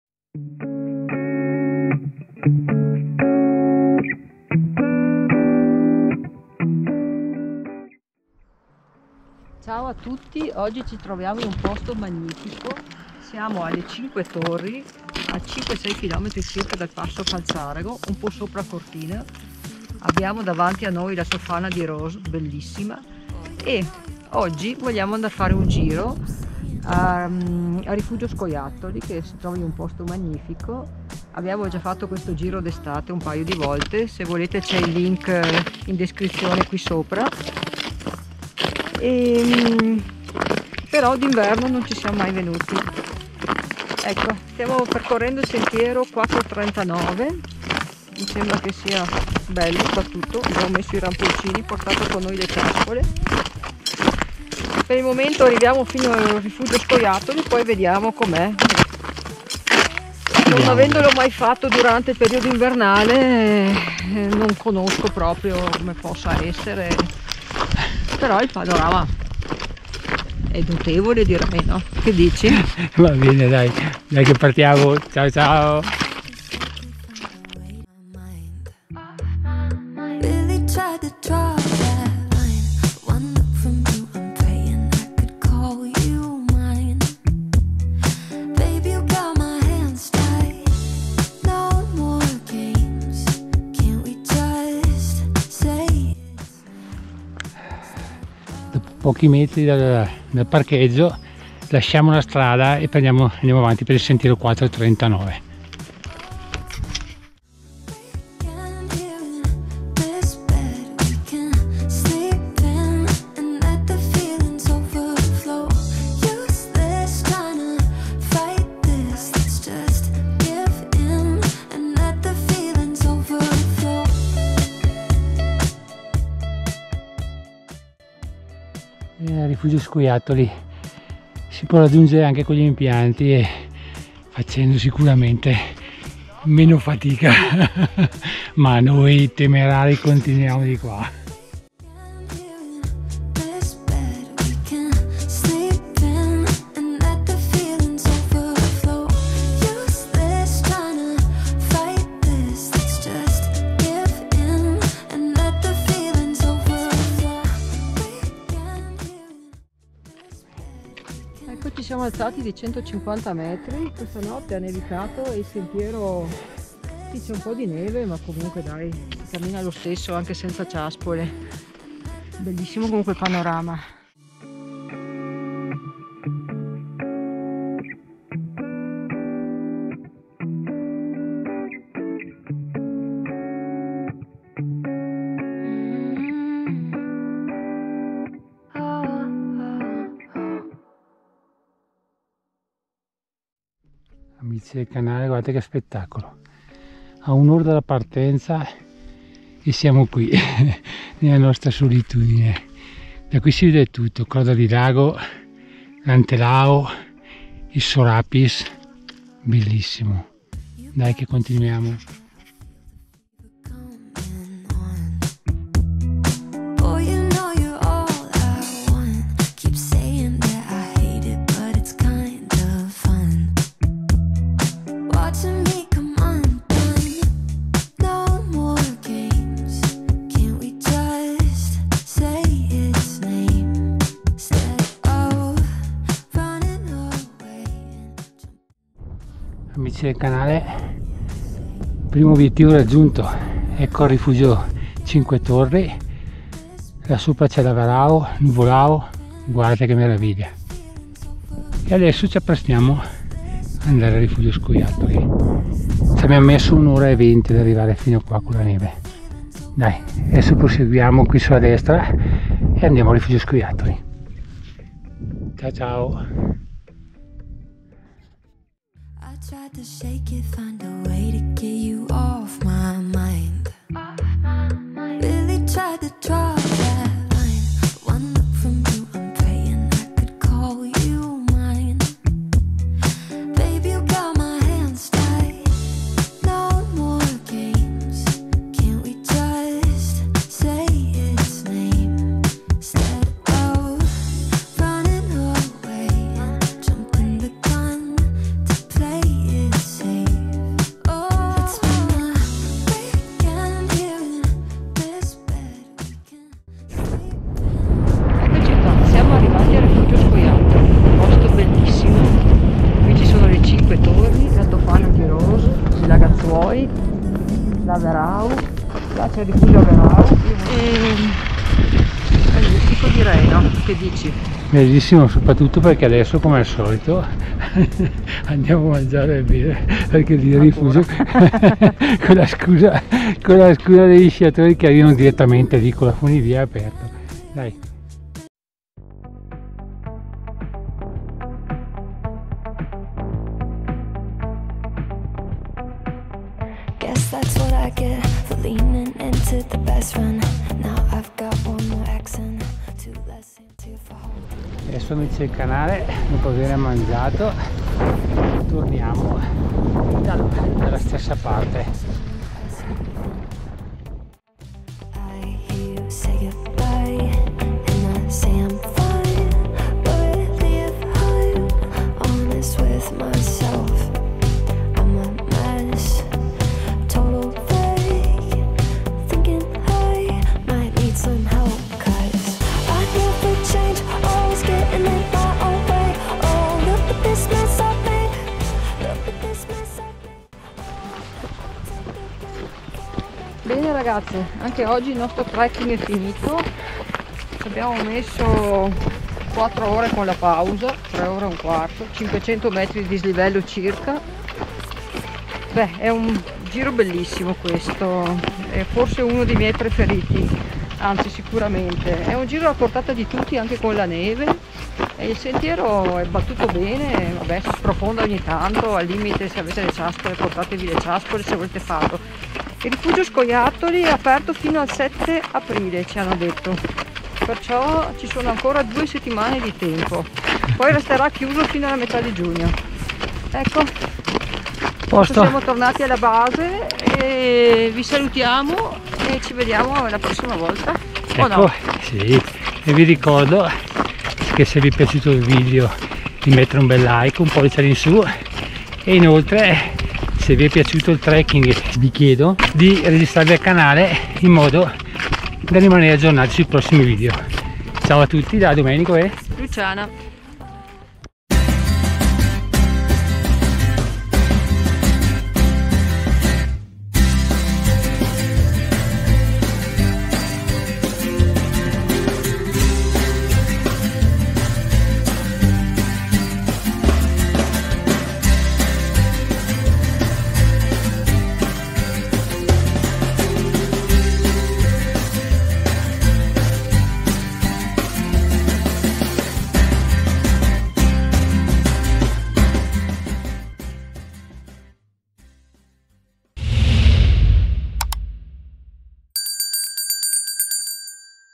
Rifugio 5 Torri | cinque Torri - Rifugio Scoiattoli - da Cianzopè Cortina
Il punto di partenza di questo trekking invernale è Cianzopè ( 1.724 mt.s.l.m), da qui, si prende il sentiero 439 che vi porterà fra incredibili paesaggi prima al Rifugio 5 Torri e poi al Rifugio Scoiattoli.

Generalmente la traccia è ben definita e battuta per la presenza di molti sciatori ed escursionisti che la percorrono.

Questo trekking invernale alle 5 Torri ( cinque torri) ti darà uno spettacolo di panorama incredibile.

La prima parte del sentiero si snoda nel bosco, appena finito il paesaggio boschivo vi si presenterà un paesaggio fantastico e indimenticabile.

In un'ora e 20 minuti nonostante la neve fresca siamo arrivati al Rifugio 5 Torri e in circa 2 ore al Rifugio Scoiattoli ( mt. 2.255 s.l.m.)

trekking estivo:Anello delle 5 Torri dal Passo Giau Rifugi Averau Nuvolau Scoiattoli e 5 Torri

00:00 Introduzione
01:17 inizio trekking
03:58 arrivo a Rifugio 5 torri
05:01 arrivo al Rifugio Scoiattoli
06:04 ritorno
06:30 conclusioni e saluti